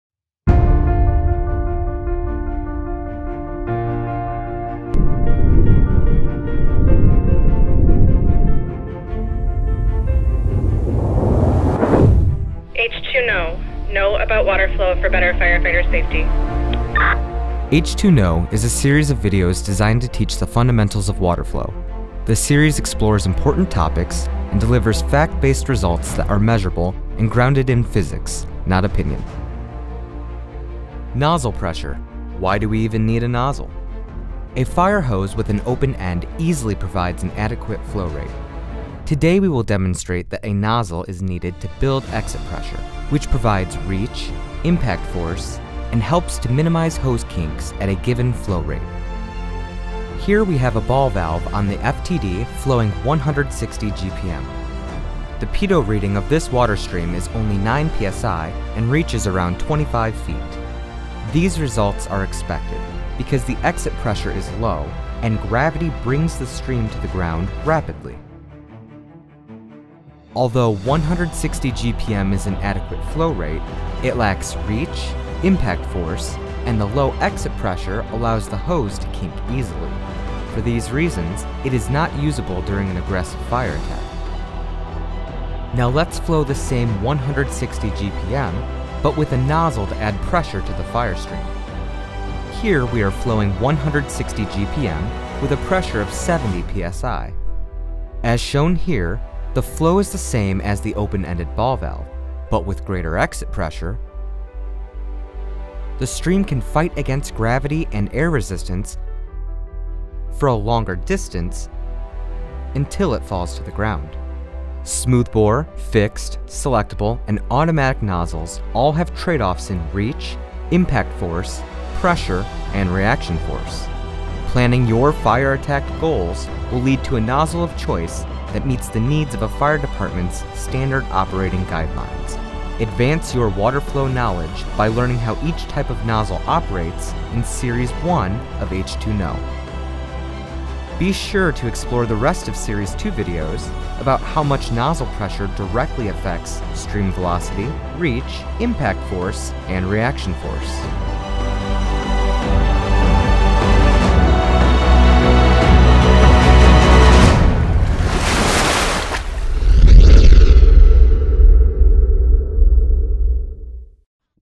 Nozzle Pressure Why Use A Nozzle Series 02 Video 01
It’s a simple question. Why do we even need a nozzle given that a fire hose with an open end easily provides an adequate flow rate? This video reviews basic, but important principles that will be useful as additional videos explore the relationship between flow, force and pressure.